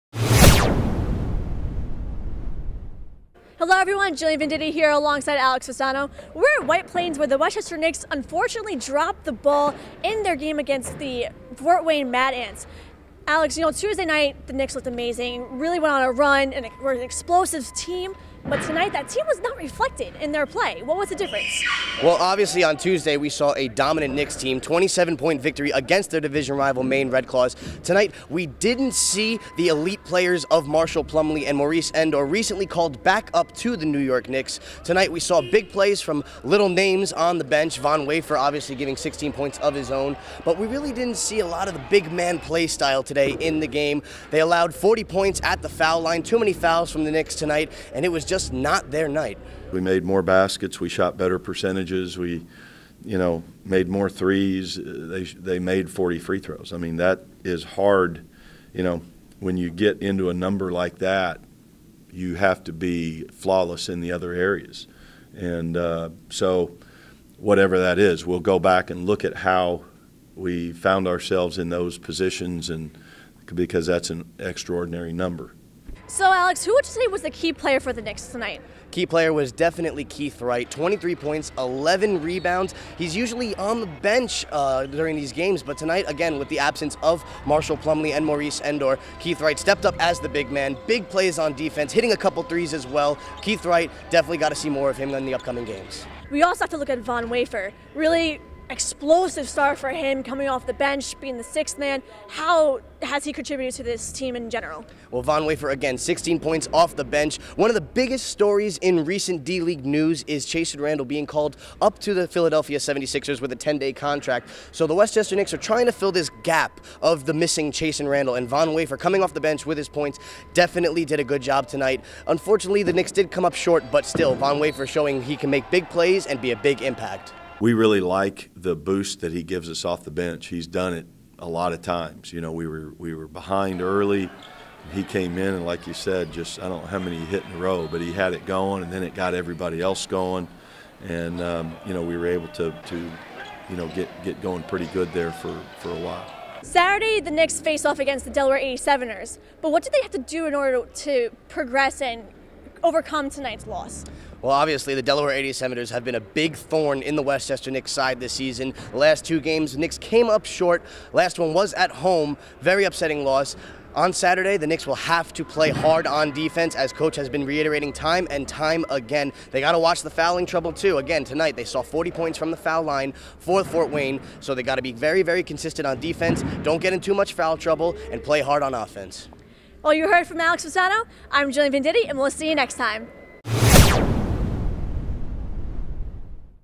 Mad Ants best the Knicks 119-111, Jillian Venditti & Alex Fasano report
WHITE PLAINS, N.Y. — The Westchester Knicks (11-11), the official NBA Development League affiliate of the New York Knickerbockers, came out on the wrong side of a hard-fought battle against the Fort Wayne Mad Ants (15-7), the Indiana Pacers’ affiliate, falling 119-111 at home. On a brighter note for the home team, 30 children from the Garden of Dreams Foundation had the opportunity to take part in special experiences throughout the game including Honorary General Manager, Honorary Westchester Knicks Dancer, and Player for the Day.

Keith Wright led the Knicks with a career-high 23 points, adding 11 rebounds for the double-double and three steals for good measure. Courtney Fells poured in 17 points with four steals three rebounds, while Doron Lamb had 15 points and four rebounds. Damien Inglis did a little bit of everything, contributing 13 points, seven rebounds, five assists, three steals, and a block. Von Wafer came off the bench for 16 points; Kevin Capers contributing 11 points and six assists.

The Mad Ants’ comeback was powered by Alex Poythress, who scored 35 points and pulled down 12 rebounds. Fort Wayne’s offensive juggernaut was continued by Trey McKinney Jones who totaled 28 total points, with 11 of those coming in the fourth quarter. Stephan Hicks put up 20 points with four crucial rebounds.

Westchester was tested early in the ballgame as they trailed nearly every minute of the opening quarter. The Knicks showed resiliency and after inching closer and closer to the lead, Jaylen Bond tied the game with 2:12 to play. Courtney Fells poured in eight points to help Westchester escape the first quarter with a 30-28 lead over the Mad Ants.

The Knicks took its hard fought lead and ran away with it in the second quarter. Westchester never trailed in the second and extended their lead as high as 13 points. Von Wafer was a huge asset off the bench leading the team with 10 points. Momentum was on the Knicks’ side as they closed out the second quarter with a 64-55 lead.

Every basket was earned in the third quarter as Westchester battled to maintain its lead over the Mad Ants. Things got too close for comfort as Westchester’s lead was cut to one with 3:17 to play. The Mad Ants were close to reclaiming its lead until Keith Wright nailed a shot from beyond the arc to help put the Knicks back up by four. At the end of three quarters, Westchester maintained an 84-80 lead over the Mad Ants.

It had been the Knicks game all night until the fourth quarter happened. After a relentless push by the Mad Ants, Westchester was outscored 39-27 in the final quarter. After the game was tied 87 a piece at the 8:58 minute mark, the Knicks’ lead was soon lost and never to be found again. Keith Wright’s instinctive play and his team-high eight fourth quarter points wasn’t enough to power Westchester past their opponent. The Knicks ultimately fell 119-111.